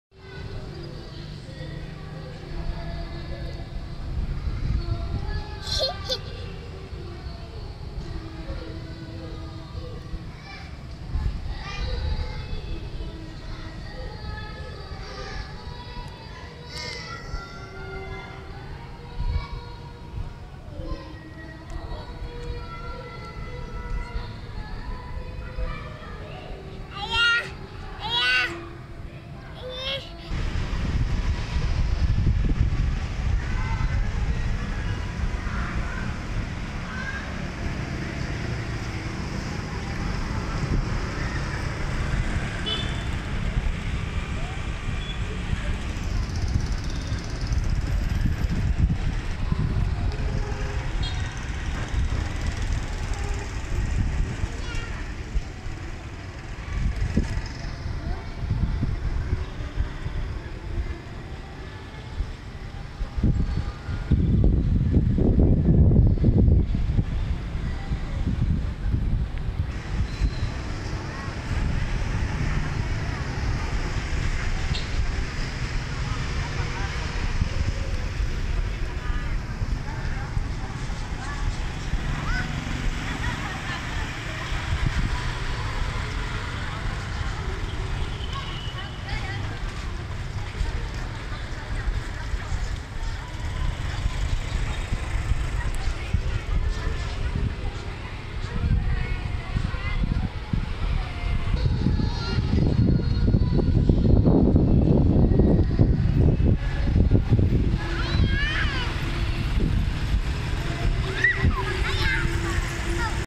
my Son is funny... Suasana Sore Hari Di Gading .. 😎😻
mau ambil moment masjid luhur di atas masjid Al barokah,, tp... tak sempurna karena kena cahaya matahari... 😭😂
mungkin kesorean ngambil video nya . . jd berpapasan dengan matahari yg mau terbenam .. hehehehheh .. lain wktu lg qta ambil moment yg pas .. 😁🙂😊🤗